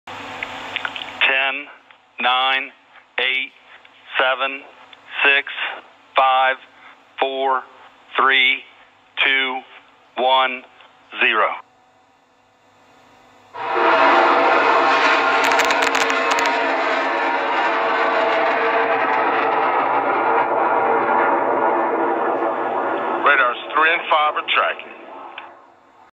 Nasa's terrier improved orion sounding launch.
A terrier improved orion  sounding rocket carrying 40 student experiments.
lift-off : 25/6/2021 8:32 a.m. edt.